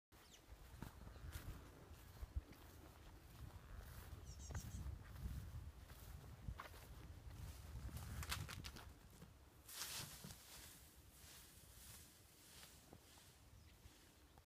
Æbletyv
Rådyrlam på æbleskud 😀